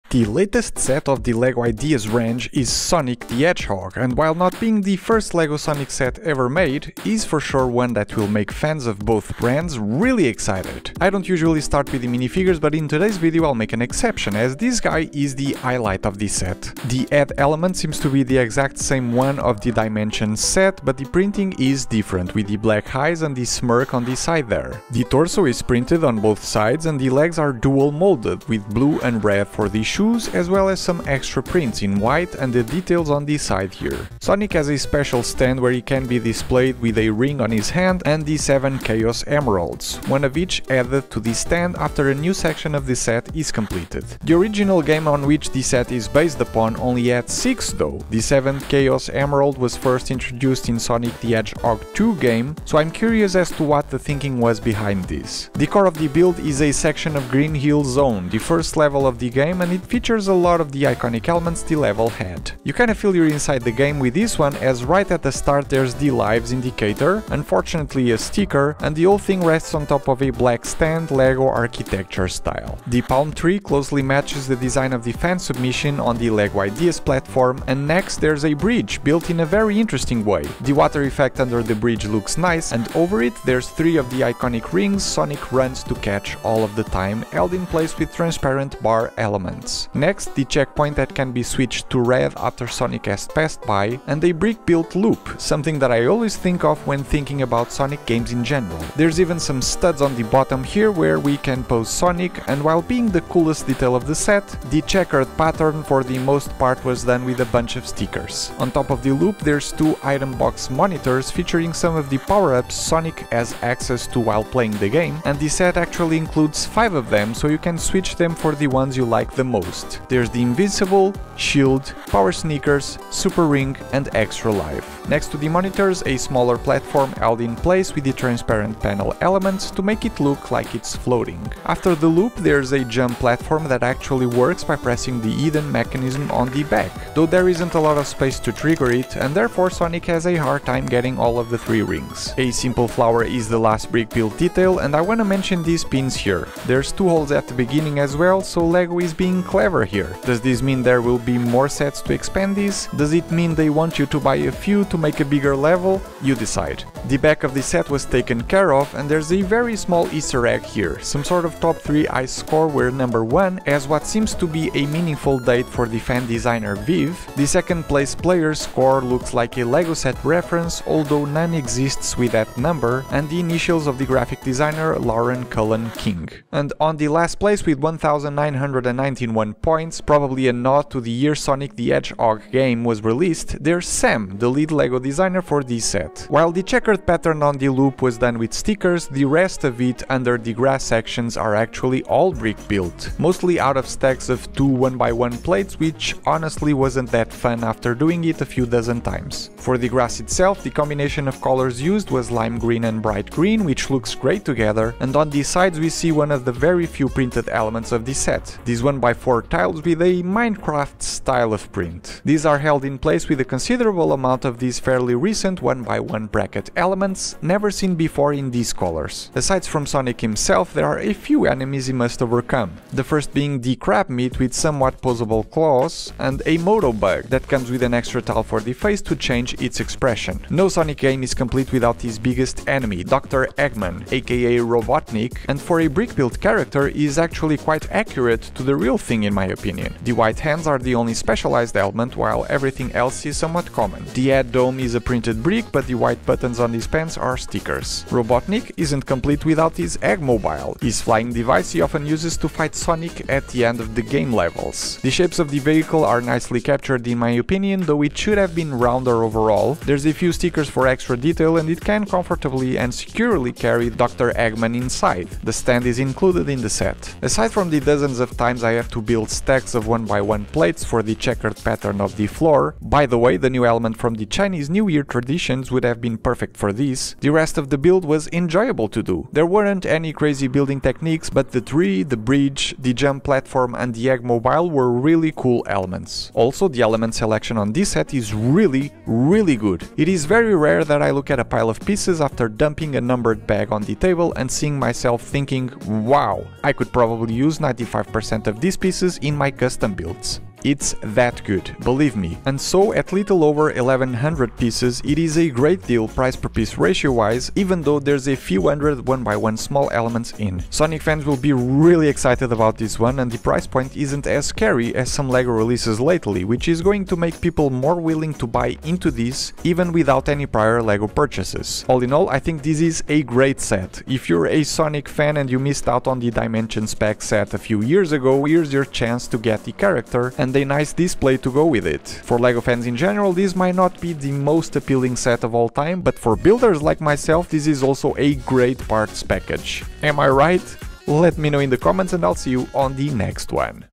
LEGO SONIC Review
In this video I'm reviewing LEGO Ideas 21331 Sonic The Hedgehog set!
A must have for fans of the game, but what about the LEGO fans?

00:00 - Intro
00:11 - Sonic
00:54 - Green Hill Zone
02:37 - Easter Eggs
03:06 - Terrain
03:39 - Enemies
04:08 - Egg Mobile
04:28 - Building Experience
05:02 - Final Thoughts

Thank you LEGO for sending this early copy to review
Views and opinions are my own

SUSBCRIBE for weekly LEGO videos!